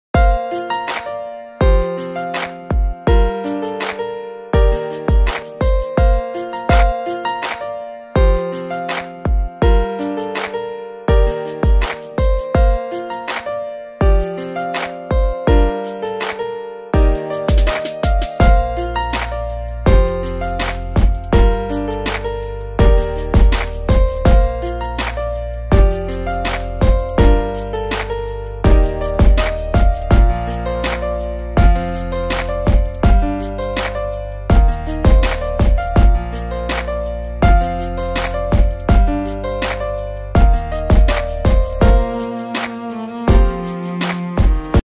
Easy recipe of chocolate souce,Used only 3 Ingredients
Easy recipe of chocolate souce,make chocolate souce at home in Very few ingredients,
Give your cake and deserts a perfect tast of chocolate with homemade chocolate souce,